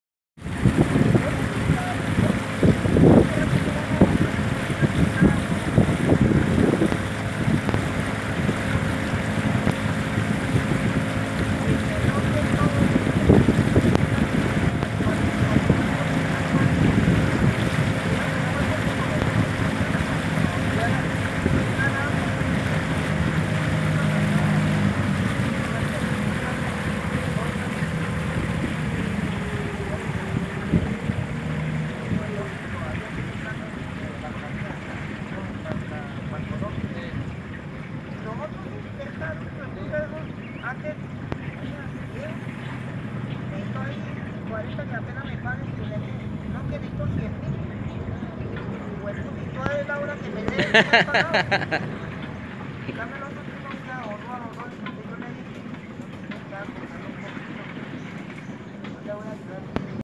Guainia afectado por la ola invernal(3)
Las inundaciones mas duras en los ultimos 42 años .